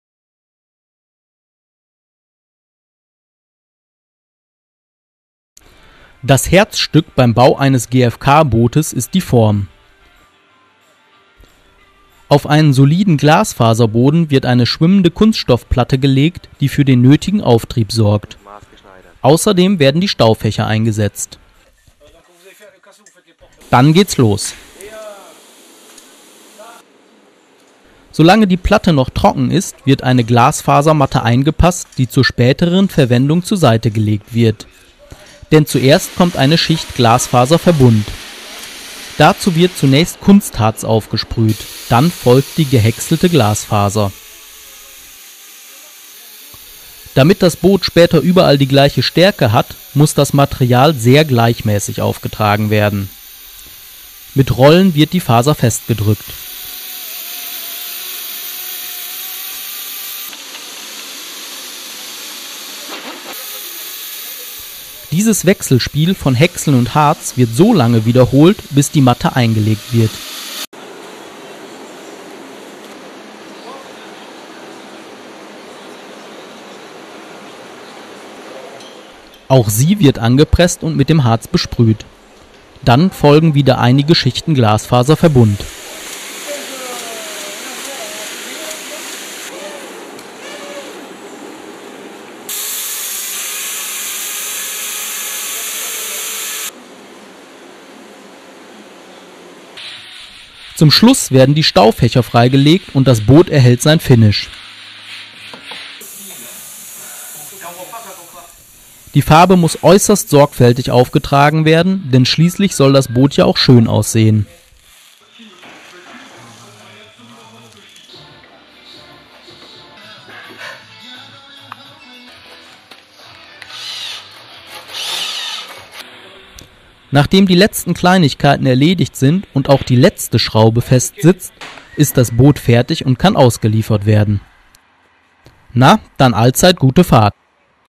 Technostrat Bootsbau "so entsteht ein Technostrat GFK Boot"
Dieses Video erklärt den Bau eines Technostrat GFK Boots